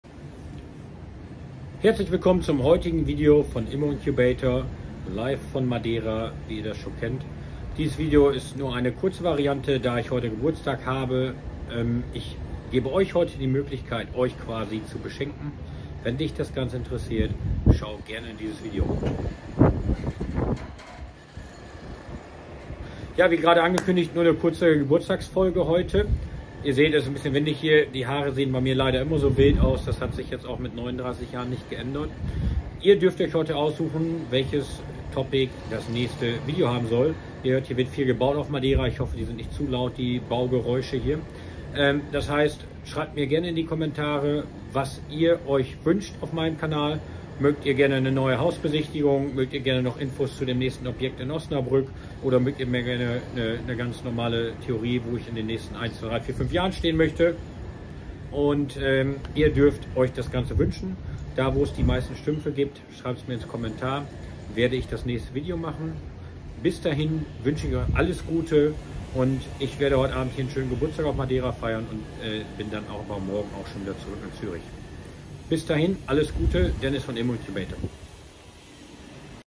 Geburtstagsvideo live aus Madeira - Mein Geschenk an Euch - Ihr dürft das nächste Video bestimmen 22
Hallo zusammen,

mein Geburtstag war am 22. Januar und deshalb habe ich nur ein kurzes Video aufgenommen. Es war etwas windig, daher sind meine Haare so hin und her geflogen.
Mein Geschenk an Euch ist das Ihr das Thema des nächsten Video bestimmen dürft.
Schreib es mir hier gerne in die Kommentare. Das Thema mit den meisten Kommentaren wird als nächstes gedreht.

Lieben Gruss
Dennis von Immoincubator